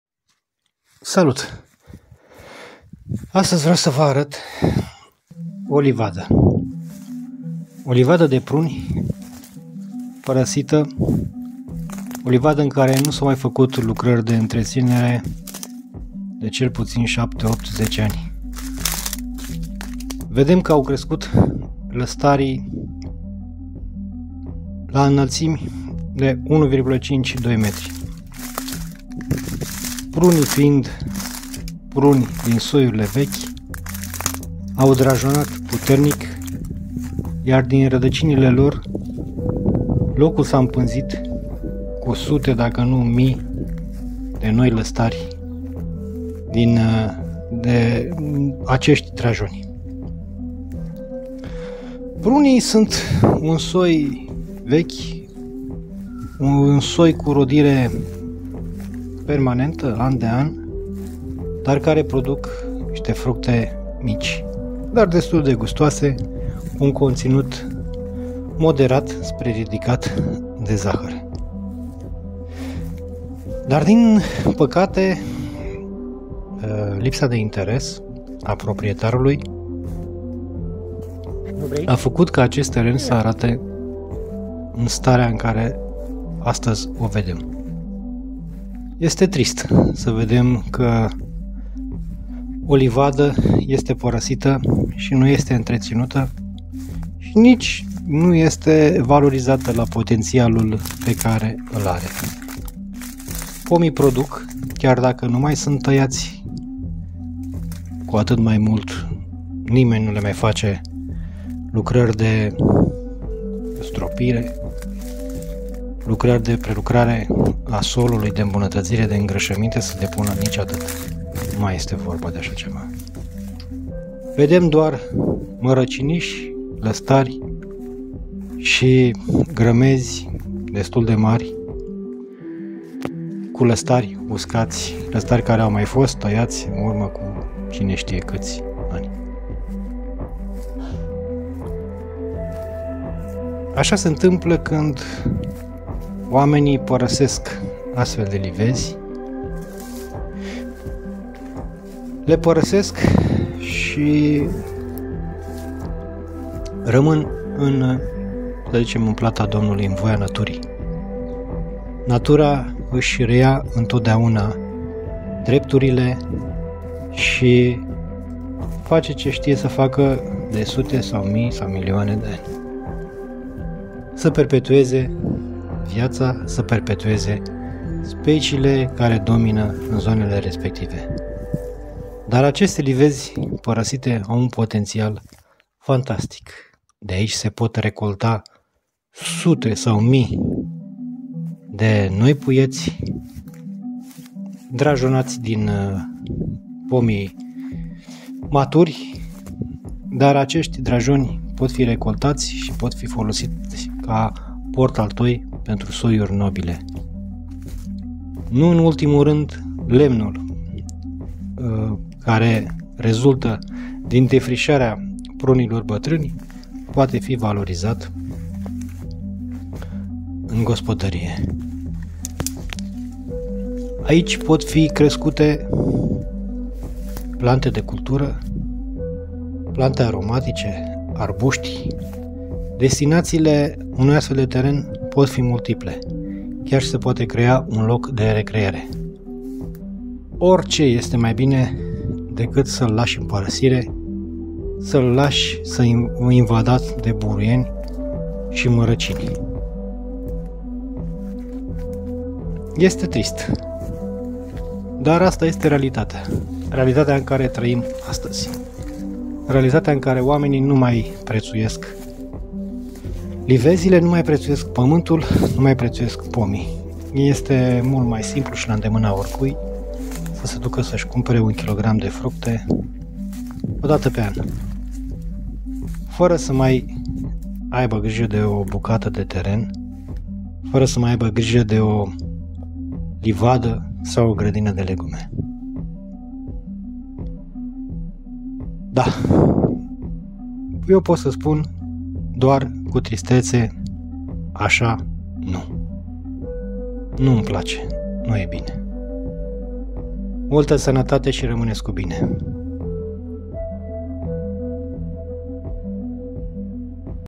Livadă de pruni părăsită.
Tot mai des livezile de pomi sunt parasite. Oamenii nu mai valorizeaza aceste bunuri si le lasa in paragina.